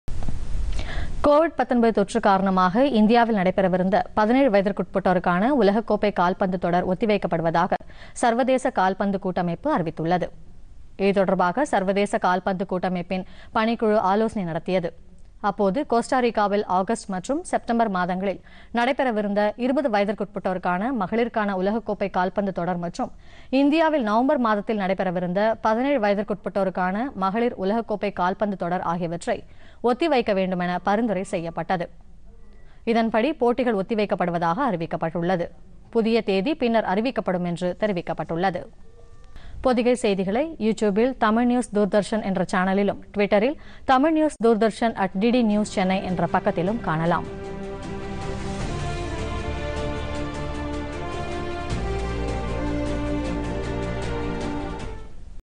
யு-17 மகளிர் கால்பந்து போட்டி ஒத்திவைப்புPodhigaiTamilNews
கொரோனா தொற்று எதிரொலி - 17 வயதிற்குட்பட்ட மகளிருக்கான உலகக்கோப்பை கால்பந்து போட்டி ஒத்திவைப்பு.Visit : TamilNews - Doordarshan